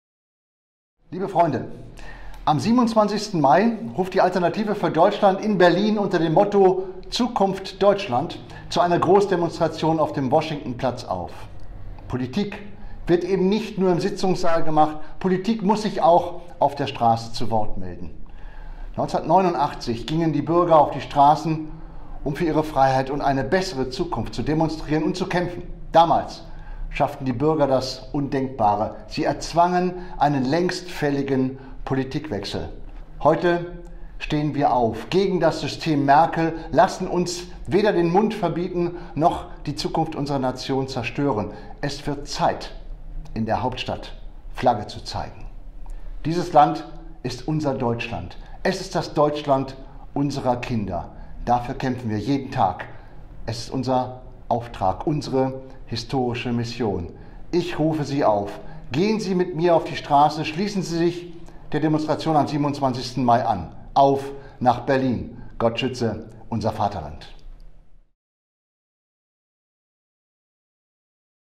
Uwe Junge - Aufruf zur Demo
AfD-Großdemonstration

am 27. Mai 2018 in Berlin
um 12 Uhr am Washingtonplatz vor dem Hauptbahnhof